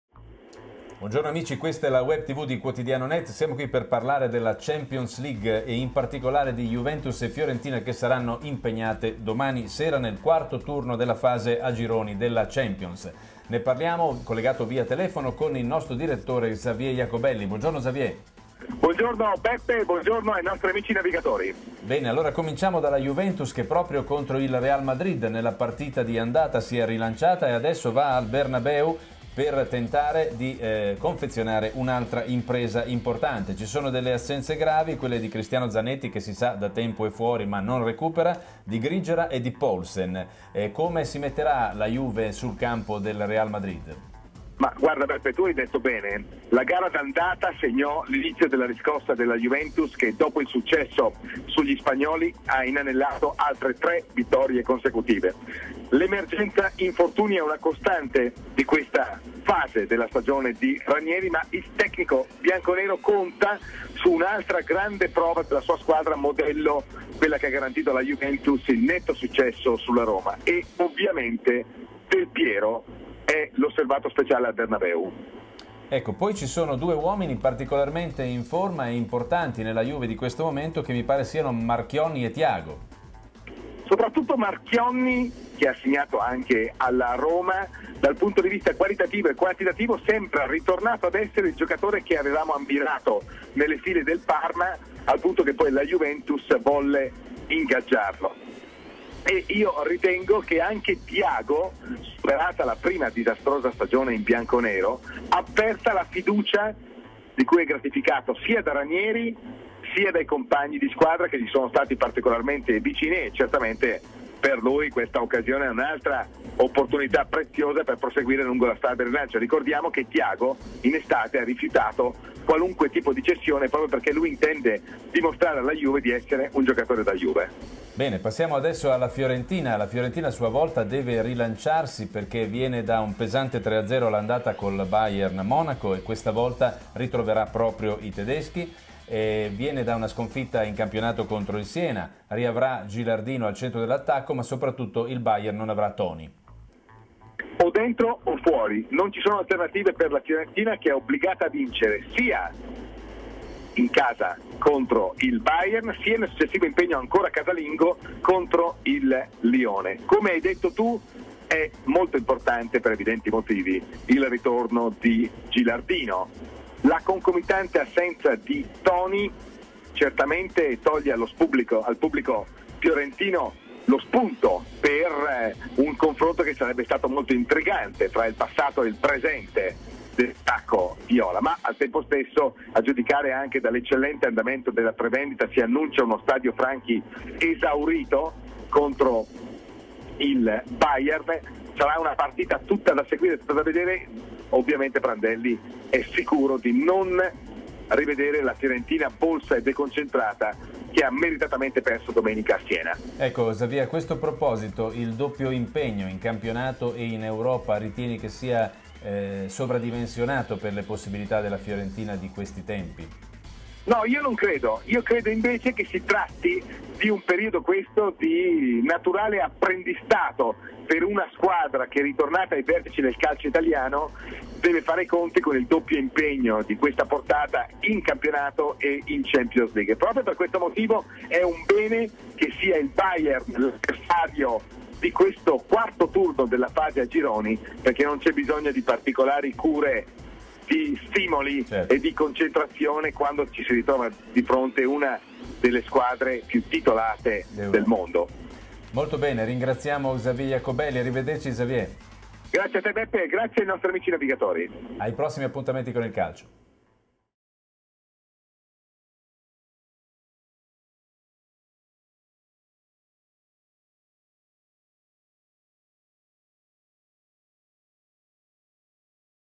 Juve nella tana del Real-Fiorentina-Bayern, serve l'impresa
Commento di Xavier Jacobelli e Giuseppe Tassi